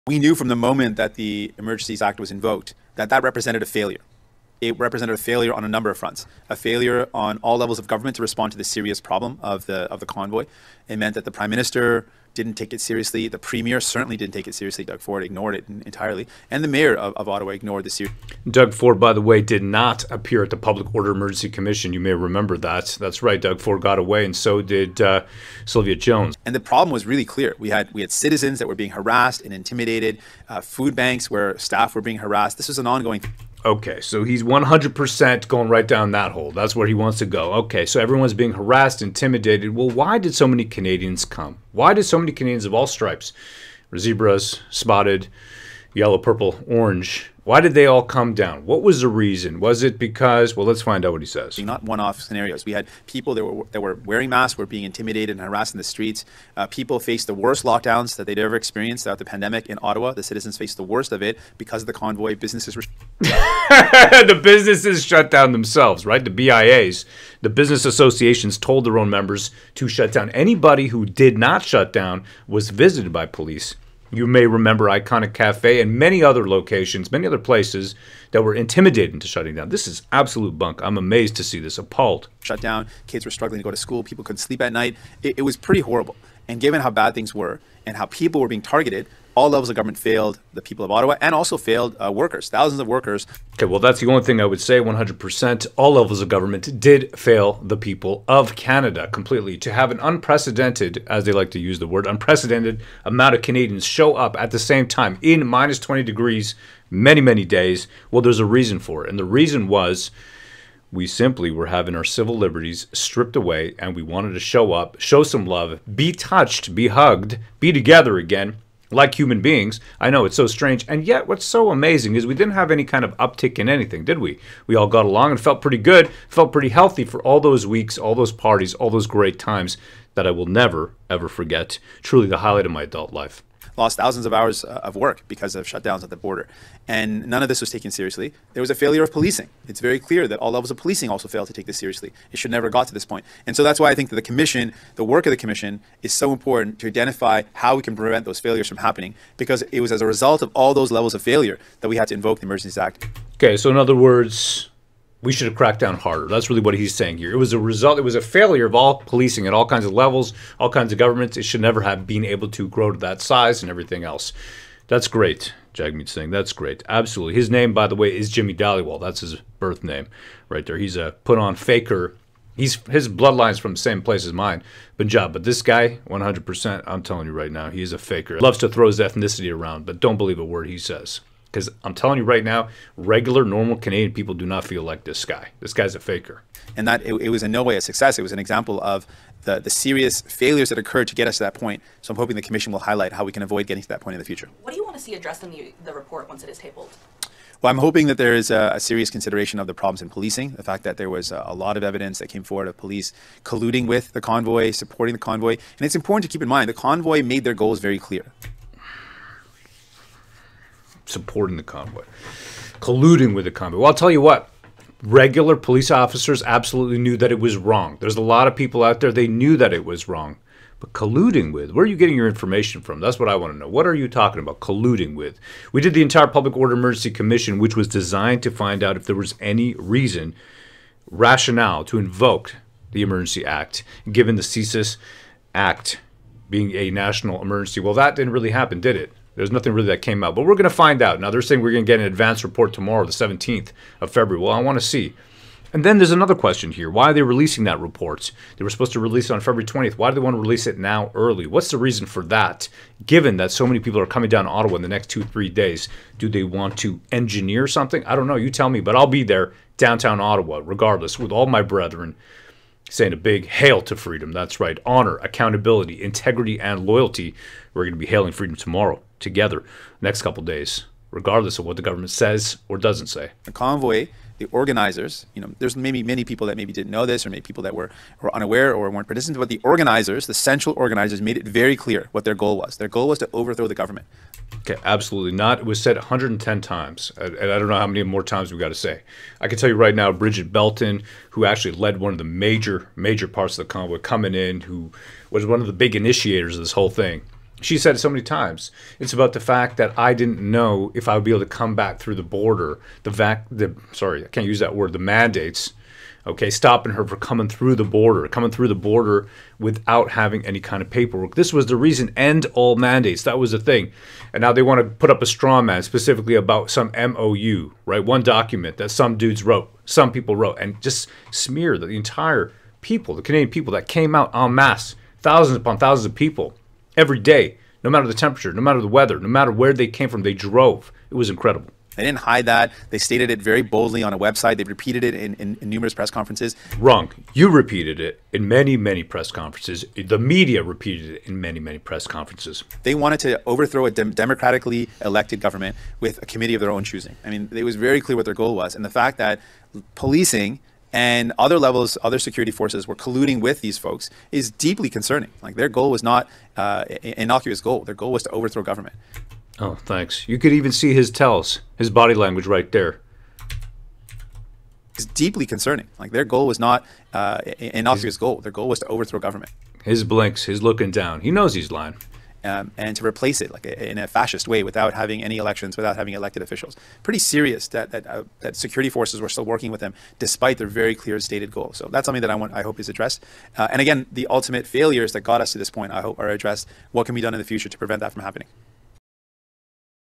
Jagmeet Singh LIES about Freedom Convoy
Jagmeet Singh NDP lies unabashedly about the Freedom Convoy Truckers Protest in Ottawa, Canada February 16, 2023